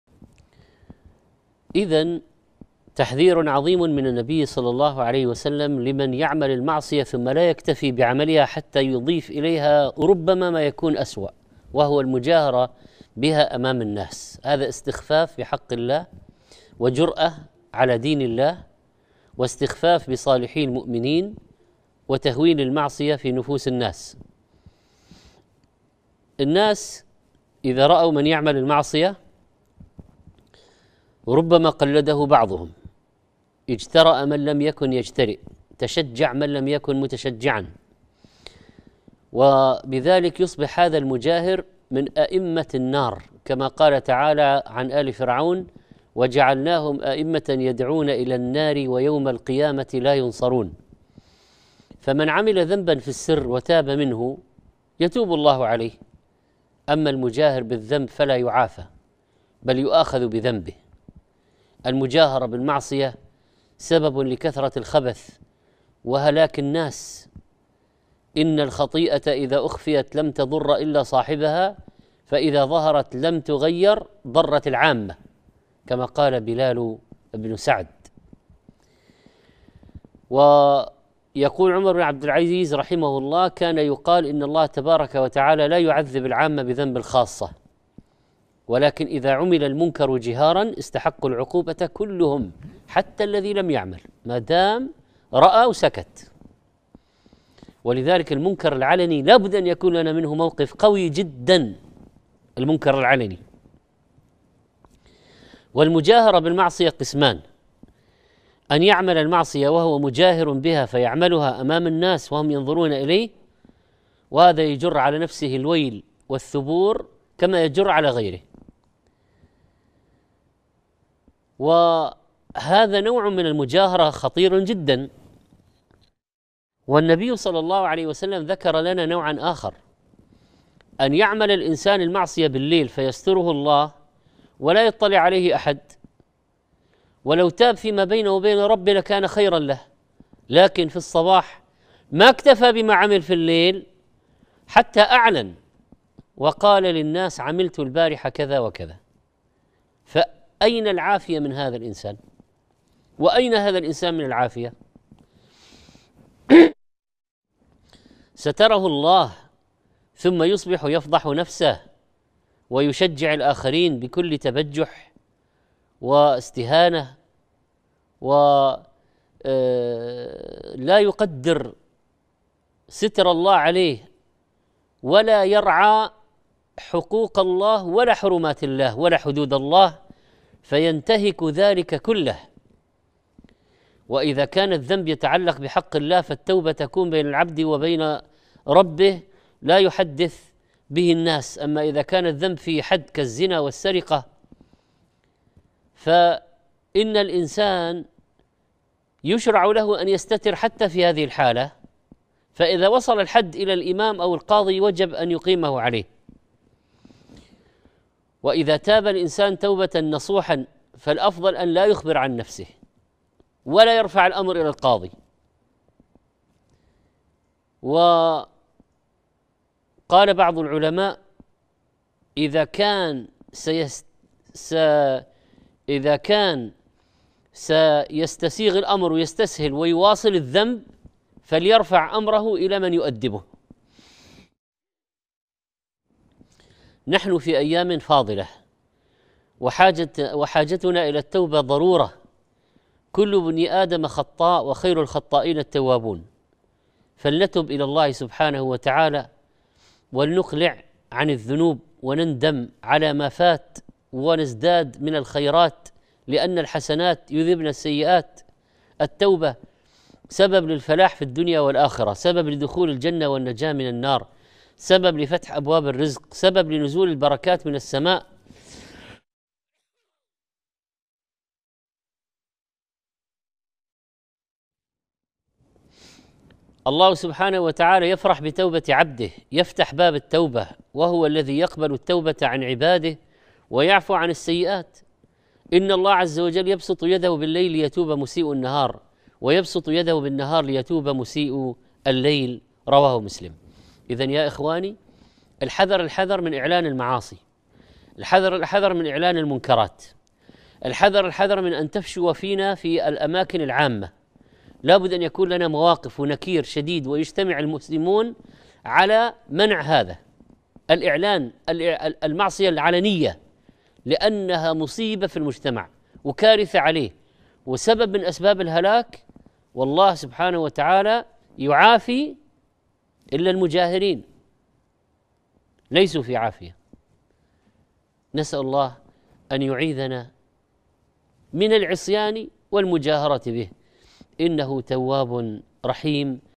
تفاخر ب المعصية قال ﷺ : كل أمتي معافى إلا المجاهرين، وإن من المجاهرة أن يعمل الرجل بالليل - المنجد
‫قال نبينا محمد ﷺ :

كل أمتي معافى إلا المجاهرين، وإن من المجاهرة أن يعمل الرجل بالليل عملا، ثم يصبح وقد ستره الله عليه، فيقول: يا فلان، عملت البارحة كذا وكذا، وقد بات يستره ربه، ويصبح يكشف ستر الله عنه